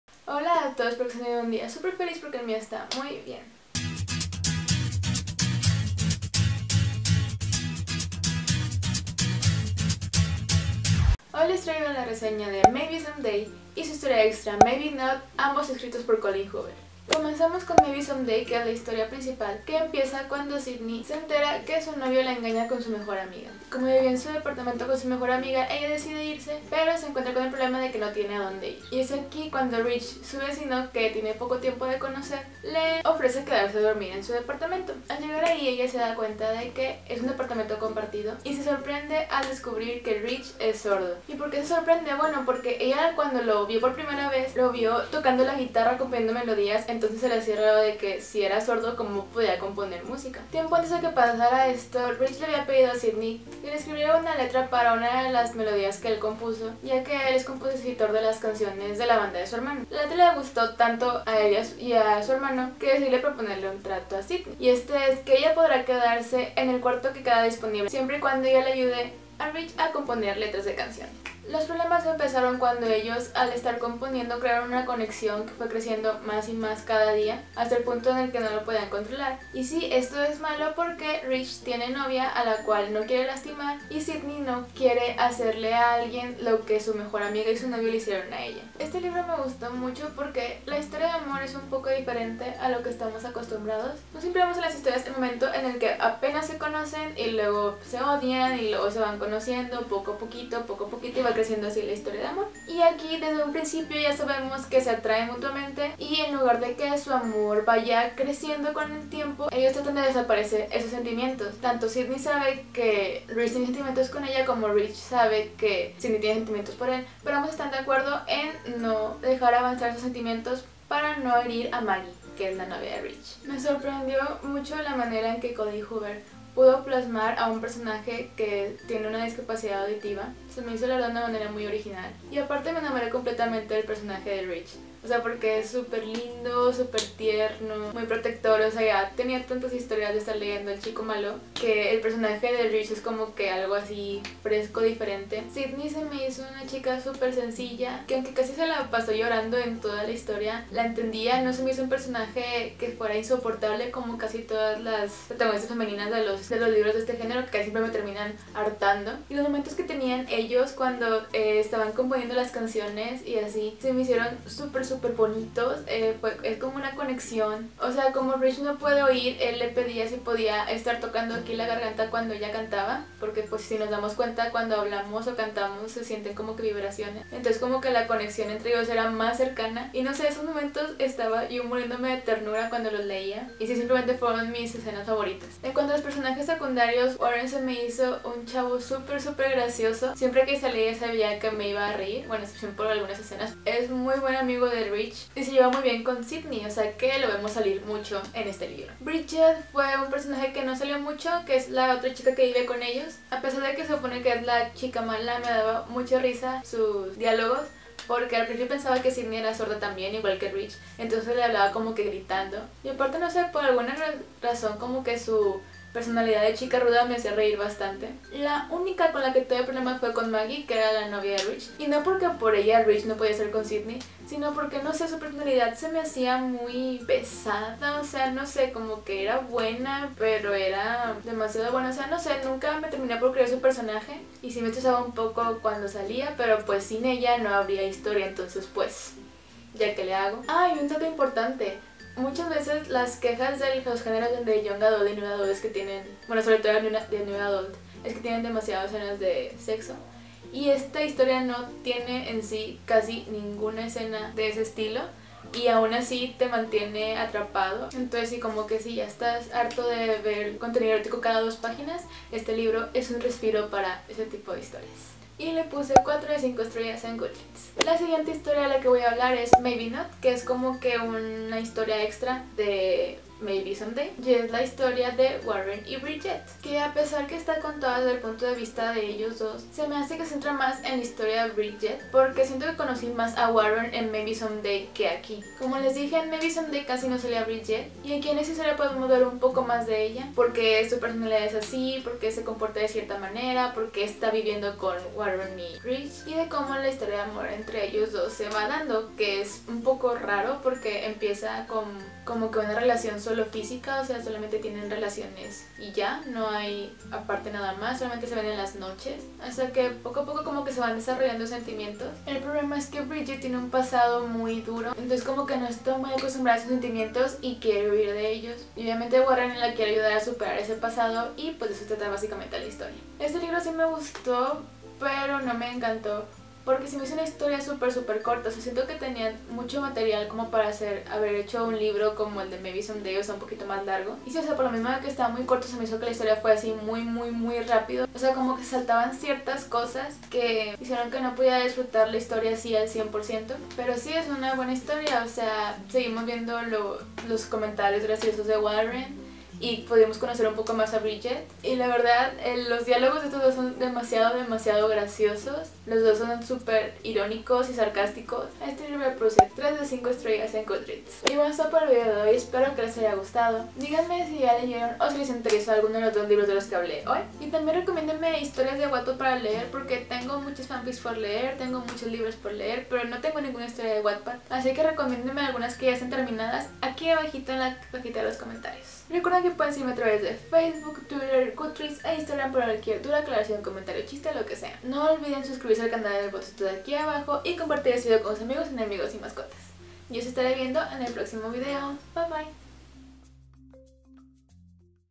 RESEÑA CON SPOILERS | Maybe Someday & Maybe Not (Colleen Hoover)
♥ CAJITA BONITA ♥
Holaaaa!! Primero que nada este es un video que debería haber subido ayer, pero empecé el gym y moría de dolor en mis piernas so.. apenas ayer tuve oportunidad de grabar :C pero bueno, hoy les traigo reseña de una historia hermosa :B que como todo libro de Colleen Hoover me encantó. ¿Que les parece a ustedes? ¿Les interesó? ¿Ya lo leyeron?

¡Abri un blog!
Aquí podrás encontrar las reseñas de lo que voy leyendo :B

Hablaremos de libros, fanfictions, películas, y más.